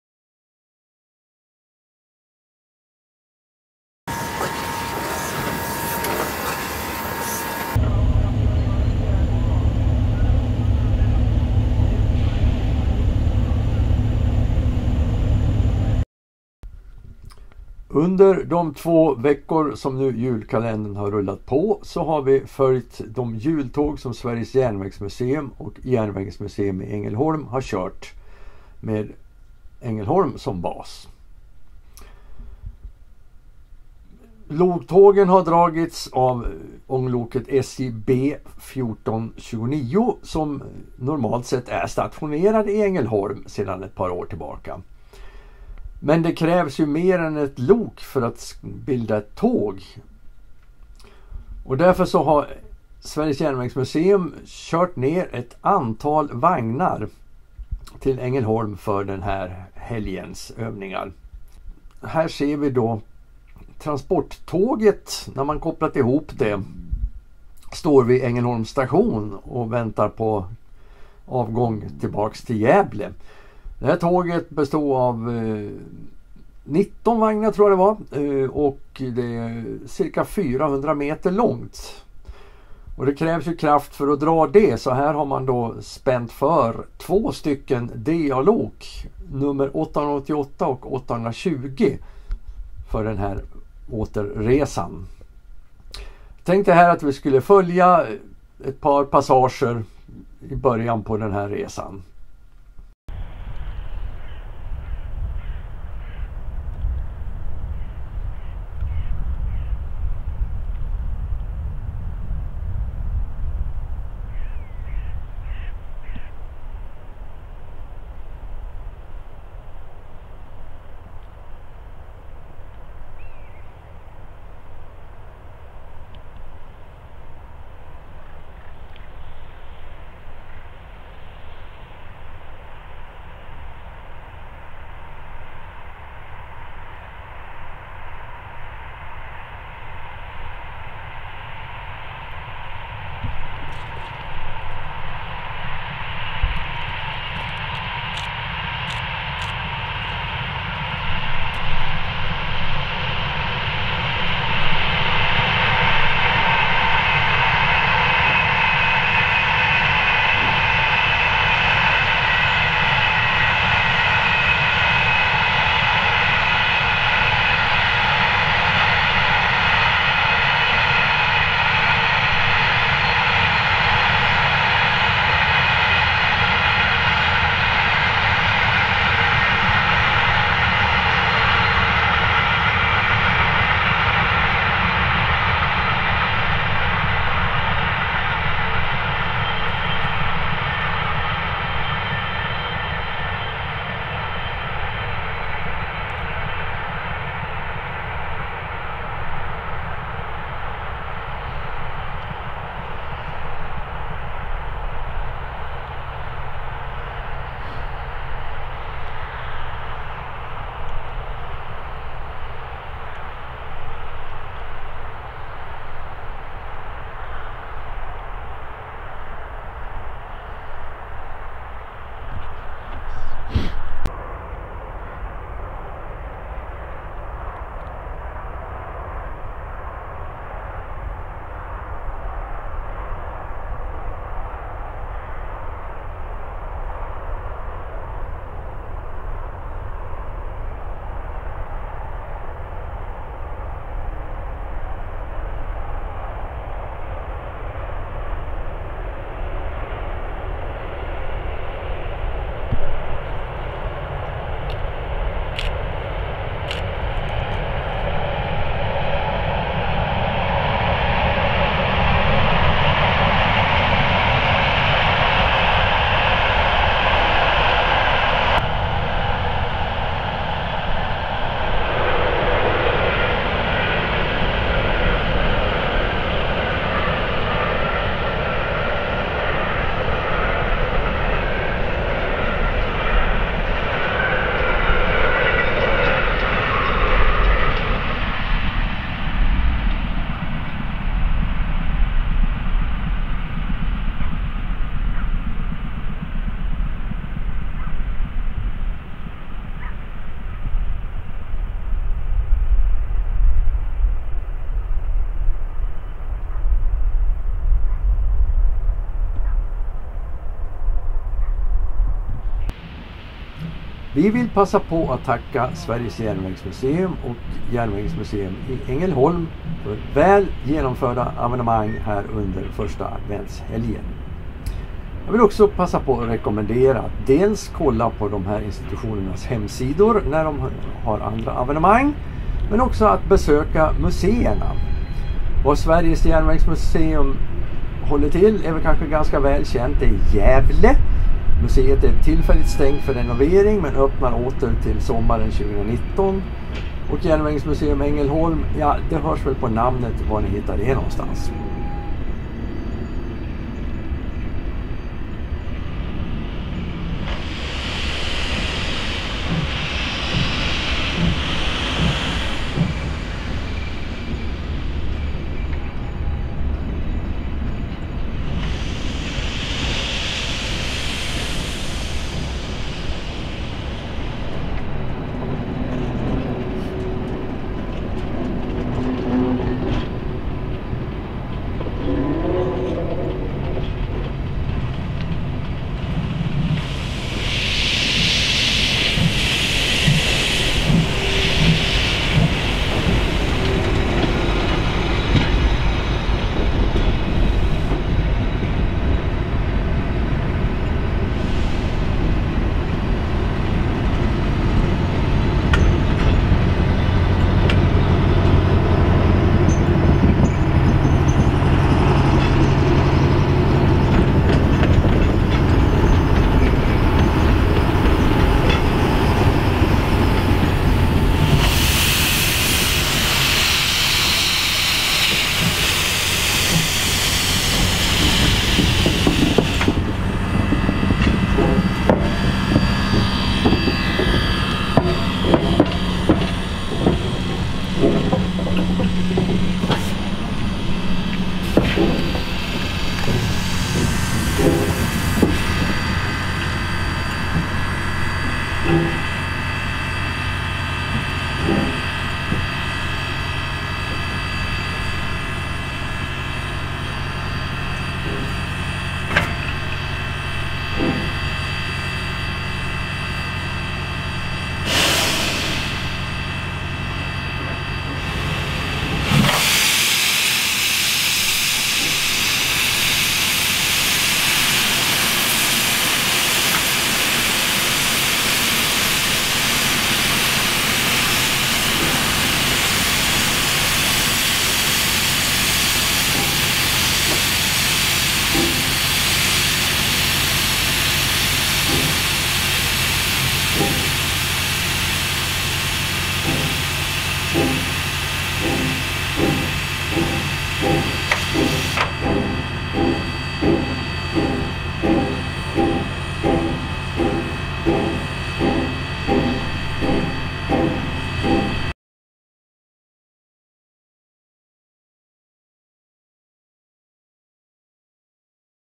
hJulkalendern 2017, 16 december KULLYWOOD
En adventskalender i form av en videoserie om hJultåg, som kördes av några olika muséijärnvägsföreningar och muséer under december 2017.
Kamera och produktion: Mats Ekberg, KULLYWOOD